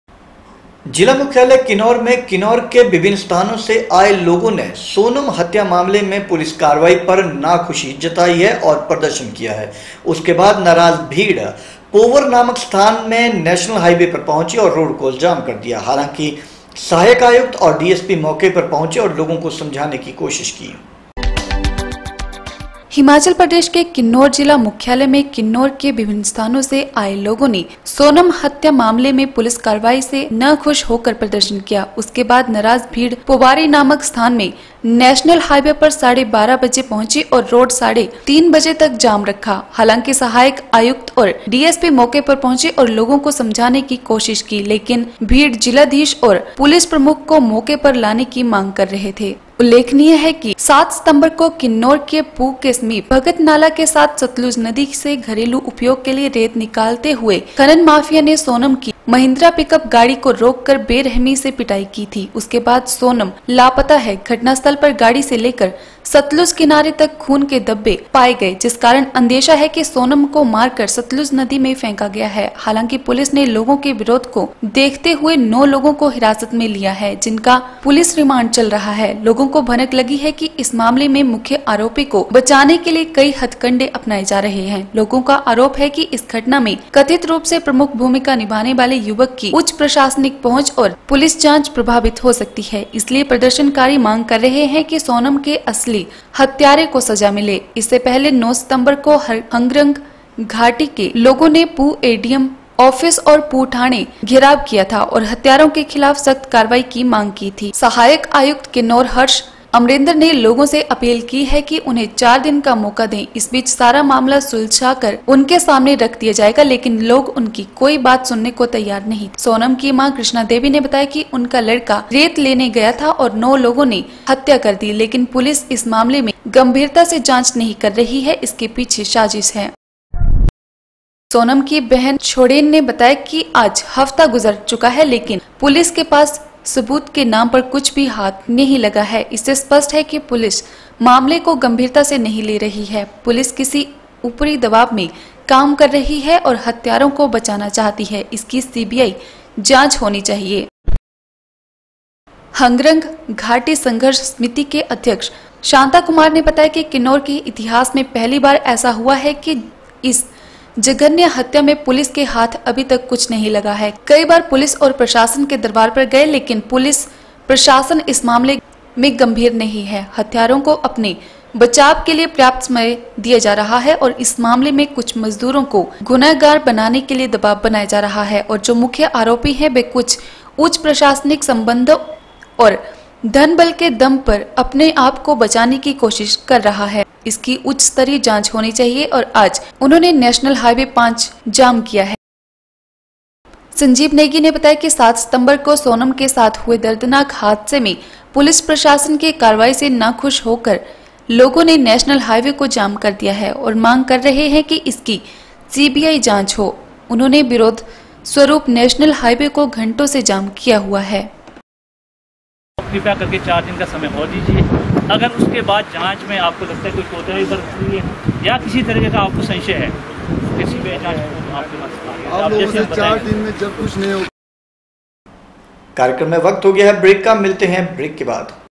16 SEP N 11 B 3 People demonstrated unhappiness with the police action in the Sonam murder case.
हिमाचल प्रदेश के किन्नौर ज़िला मुख्यालय में किन्नौर के विभिन्न स्थानों से आये लोगो ने सोनम हत्या मामले में पुलिस कारवाही से नाखुश हो कर प्रदर्शन किया। उस के बाद नाराज भीड़ पोवारी नामक स्थान में नेशनल हाइवे पर पहुँची और रोड  जाम रखा। हालांकि  सहायक आयुक्त और डीएसपी मौके पर पहुँचे और लोगो को समझाने की कोशिश की